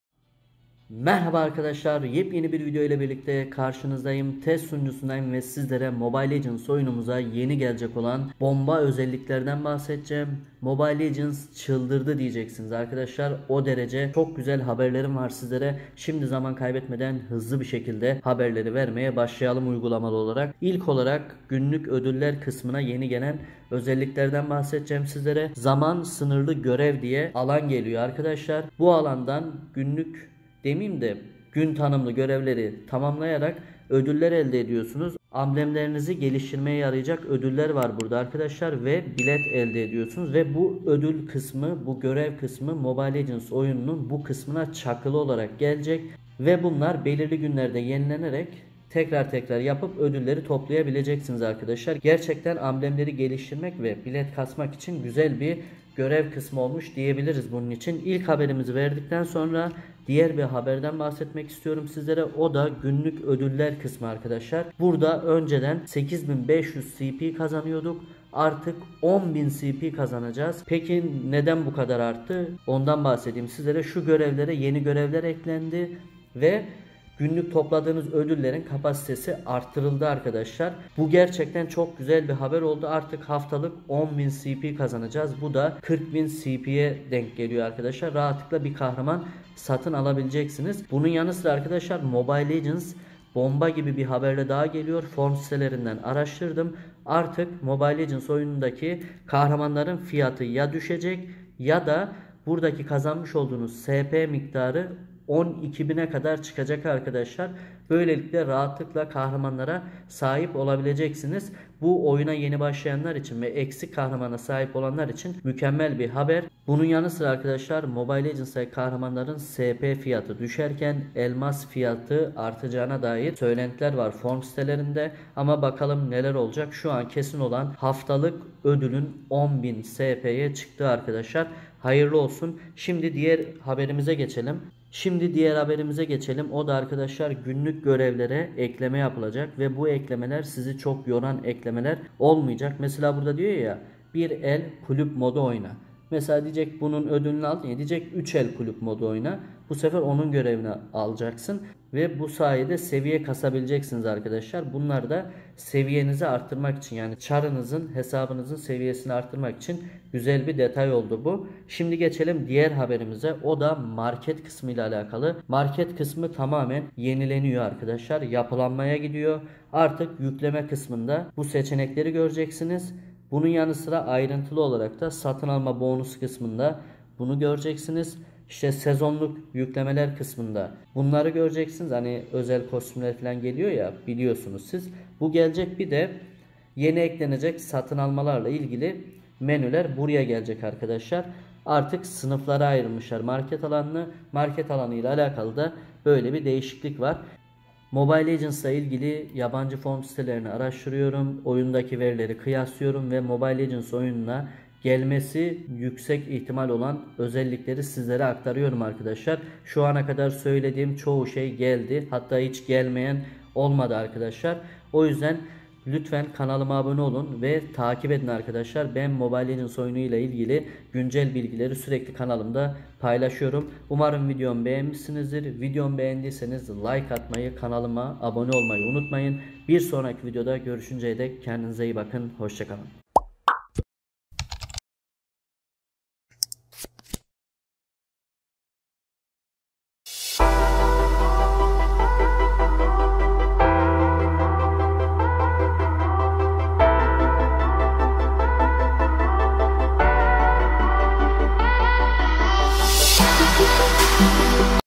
MLBB ÇILDIRDI! Haftalık 10Bin SP | Yenilenmiş Market ve Sınırlı Görevler | Mobile Legends Bang Bang
Haftalık daha fazka SP kazanacaksınız, market alanı değişiyor ve günlük görevlere eklemeler var. Detaylar videoda...
Discort Sunucumuza Katılın Birlikte Eğlenelim!
Sizleri çok seviyorum TEŞEKKÜRLER ❤🙏